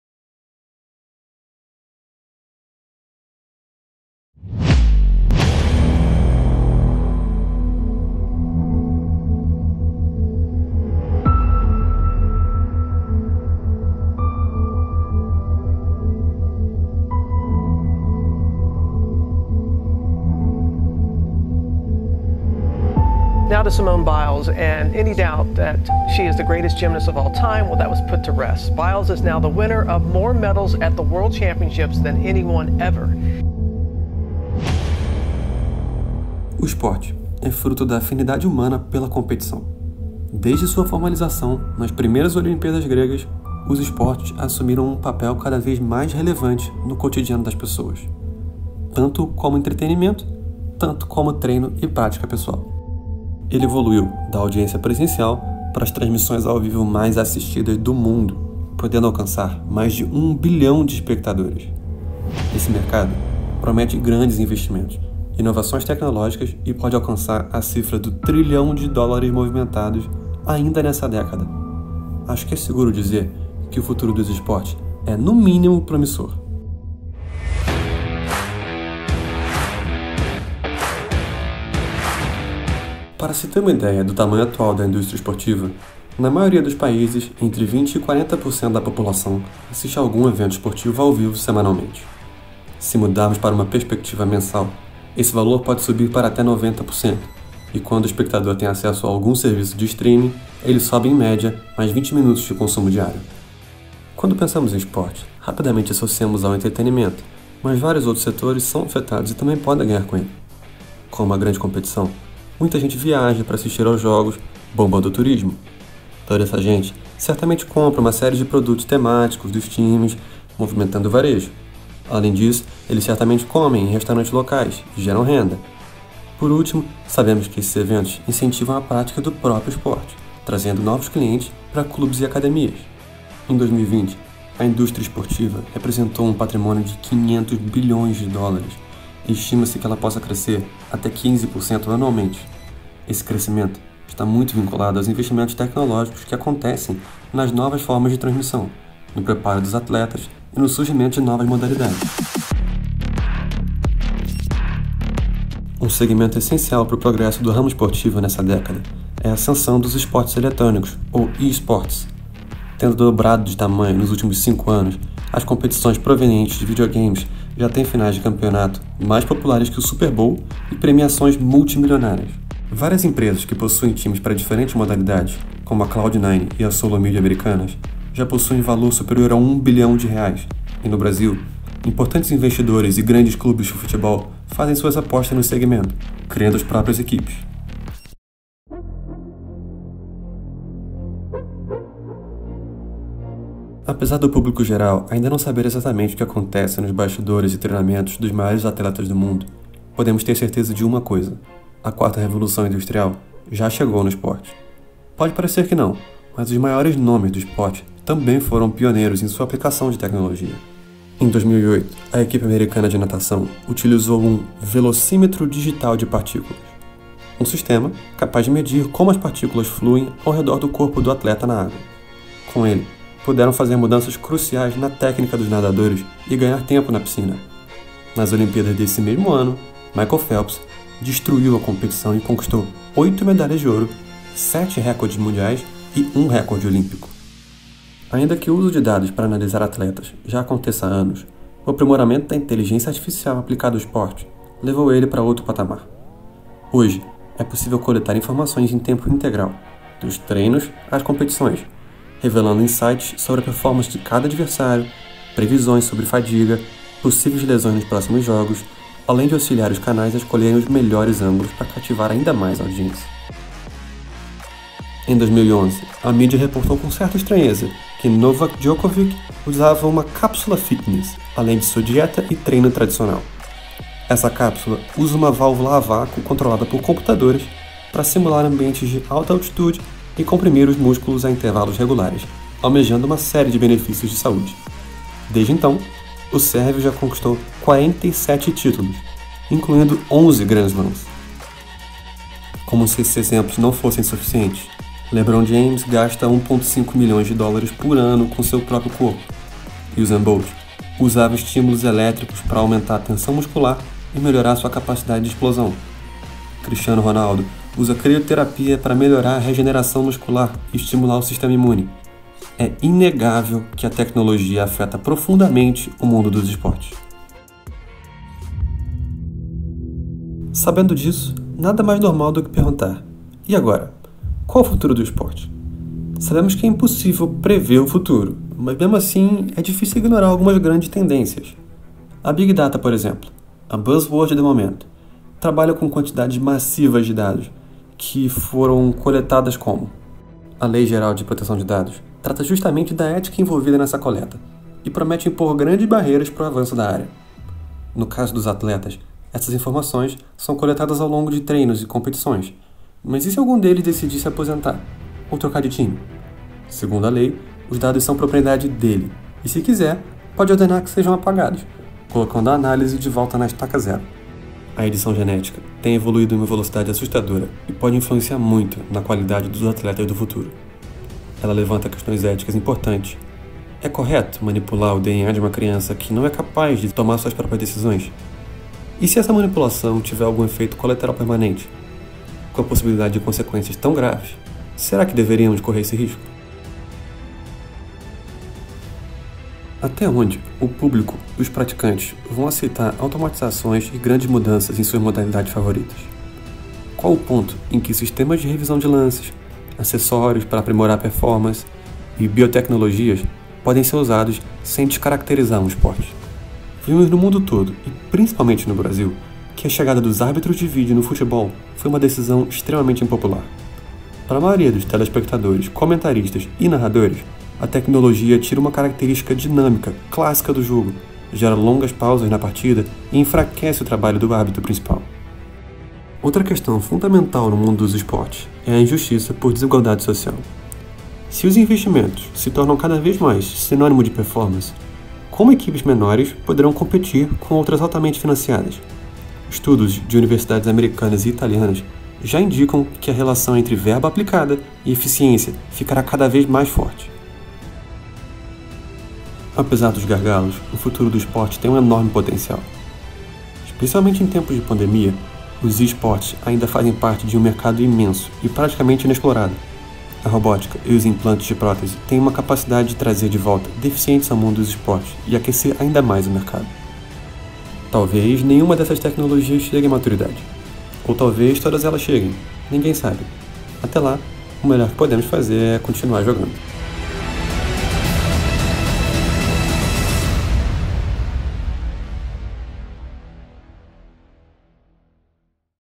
A História da Tecnologia no Esporte
Esse vídeo estabelece uma relação entre passado, presente e futuro da tecnologia no esporte. Apresenta também os gargalos tecnológicos, sociais, políticos, econômicos, culturais, regulatórios presentes na evolução do esporte.

Neste vídeo, é parte dos trabalhos desenvolvidos na disciplina Historia da Tecnologia da Universidade Federal do Rio de Janeiro (UFRJ).